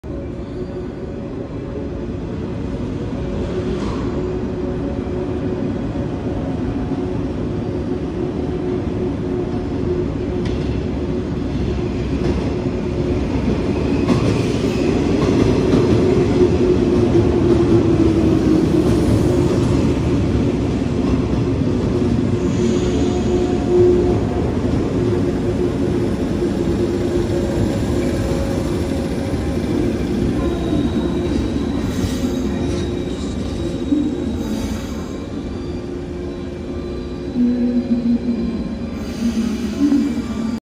115系回送発車115系山陽本線普通相生行き到着 岡山駅にて
岡山ドームに程近い岡山駅。1番のりばから発車する115系6両編成回送。当駅まで姫路始発、普通岡山行きであった。3番のらばに到着する115系（A編成＋D編成）7両編成。8時7分発普通相生行き。